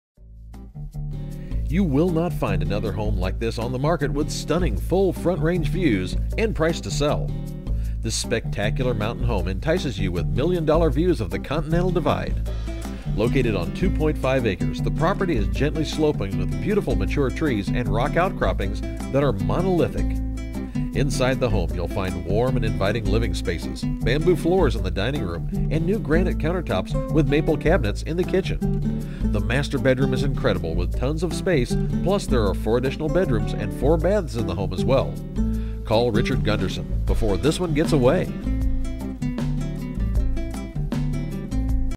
11950 Spruce Canyon Cir Golden, CO Homes for Sale | coloradohomes.com
11950 Spruce Canyon Cir, Golden, CO
 Richard Gundersen-Coldwell Banker |

Spectacular mountain home has million dollar views of the Continental Divide! Located on 2.5 acres, the property is gently sloping with beautiful mature trees and rock outcroppings that are monolithic! This 5 bedroom, 4 bath home features bamboo floors in the dining room, new granite counter tops with maple cabinets in the kitchen. The master bedroom is incredible with over 400 square feet. 1,760 sf garage. Not another home on the market with stunning full Front Range views and priced to sell!